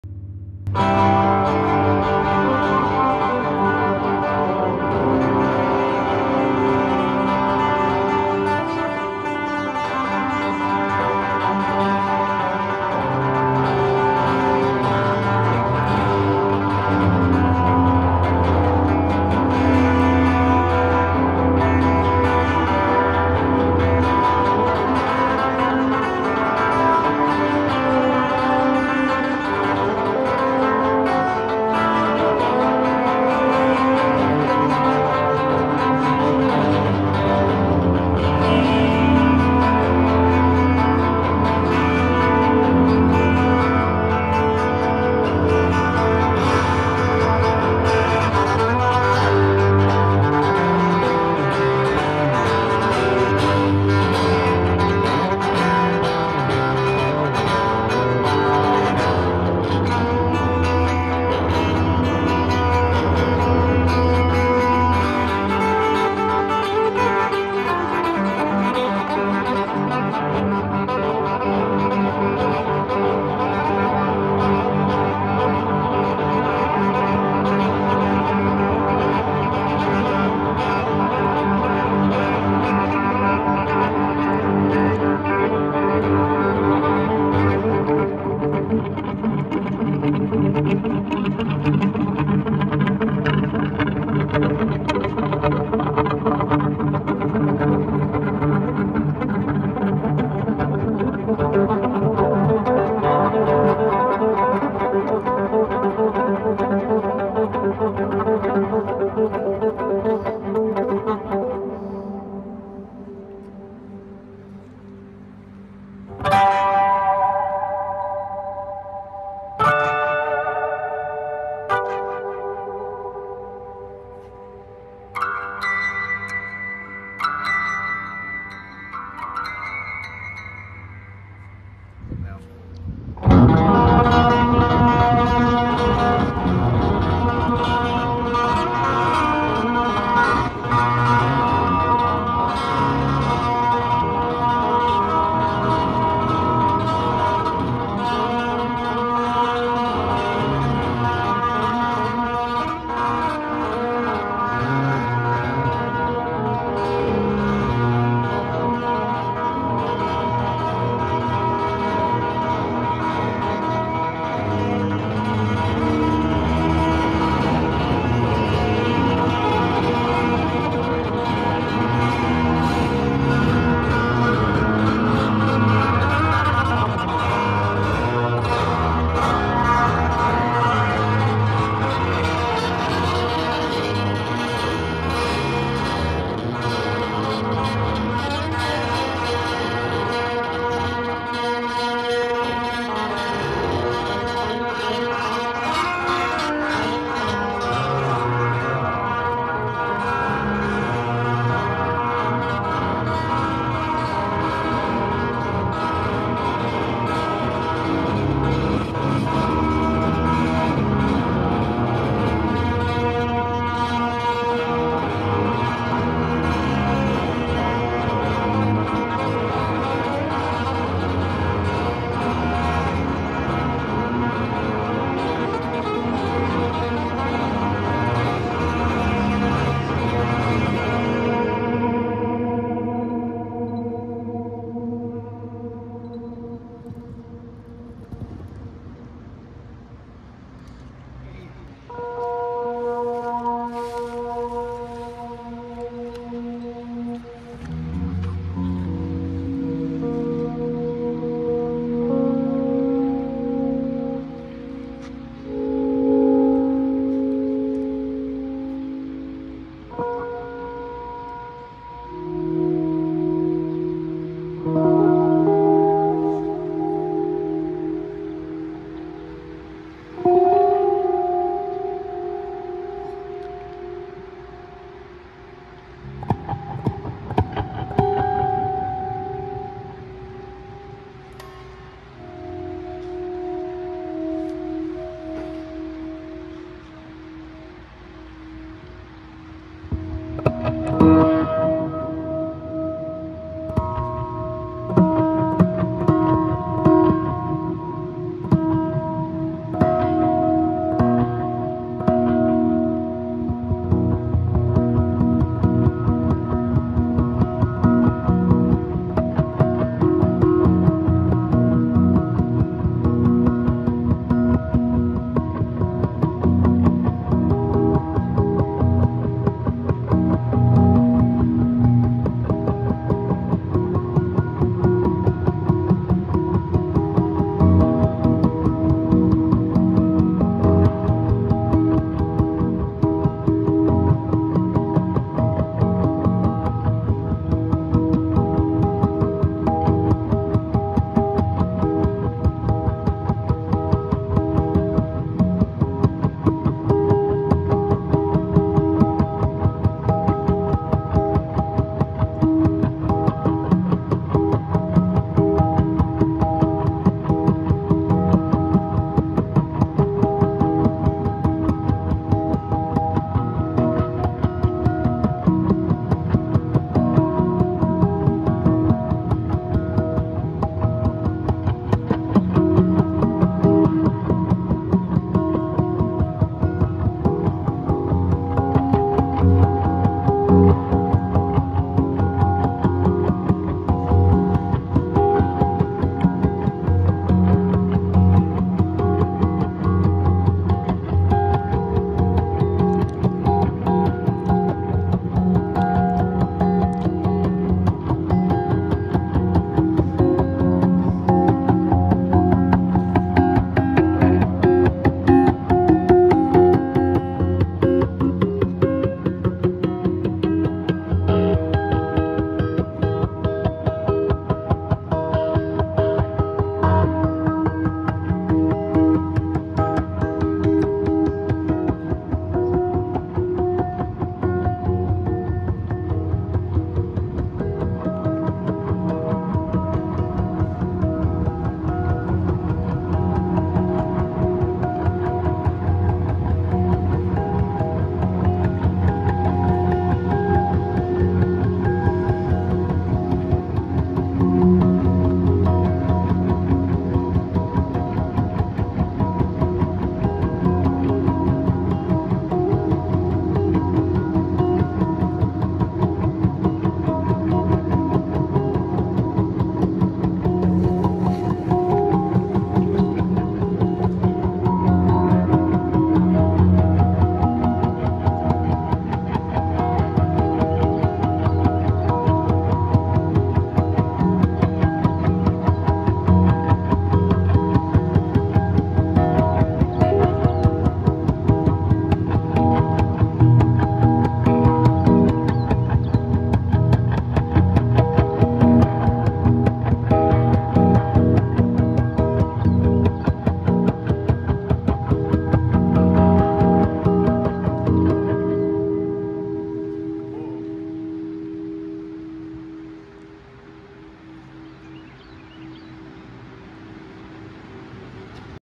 Jonathan Horne, sounding the echo bridge
live, no effects

on the san antonio river